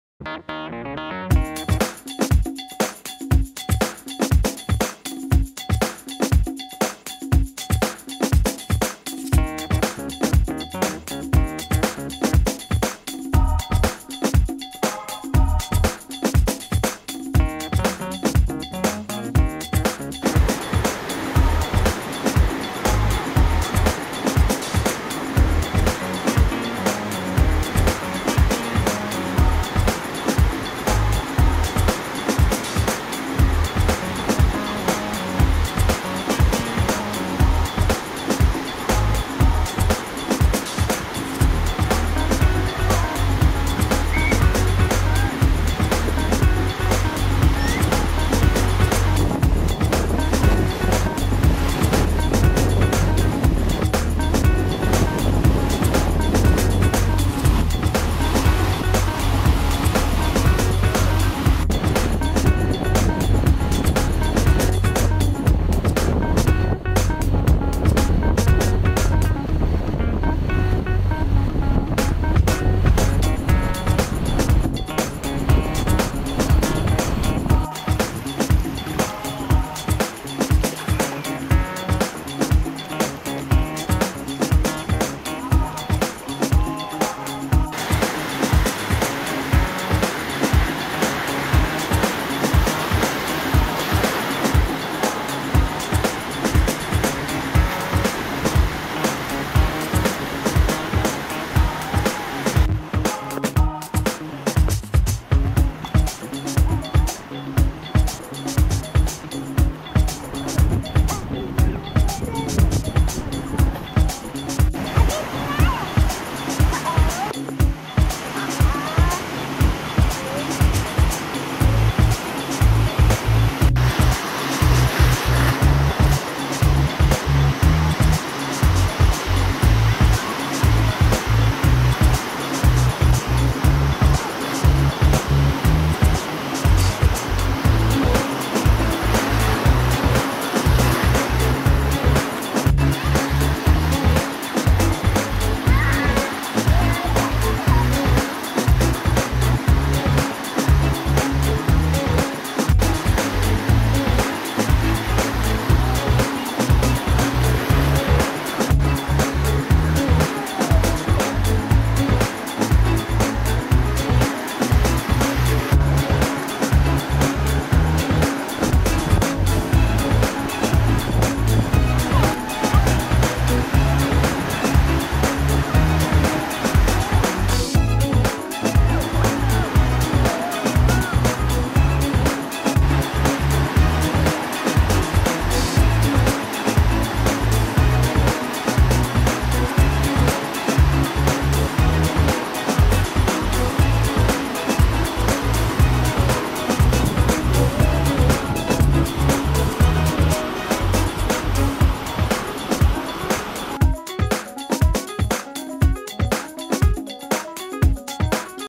5 year old surfer surfing Byron Bay - Scott Arderne
Little Scott - Surfing the Pass at Byron Bay on his new 4ft 10inch Dorrington. Got his new board for his birthday and some fun waves to go with it.